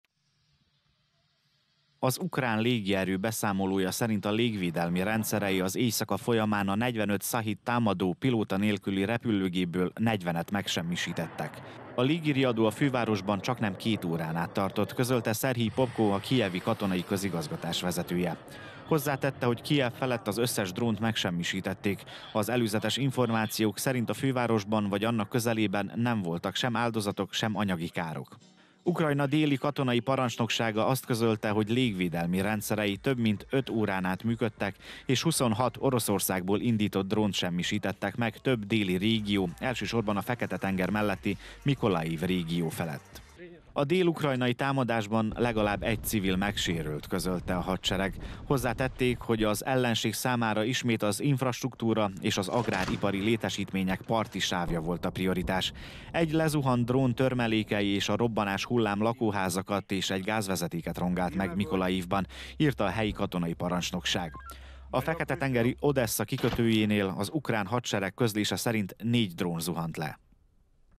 2024.02.11. - Újabb dróntámadás ukrán célpontok ellen
Oroszország ismét dróntámadásokat indított Kijev és Dél-Ukrajna ellen, legalább egy civil megsebesült és egy gázvezeték is megsérült.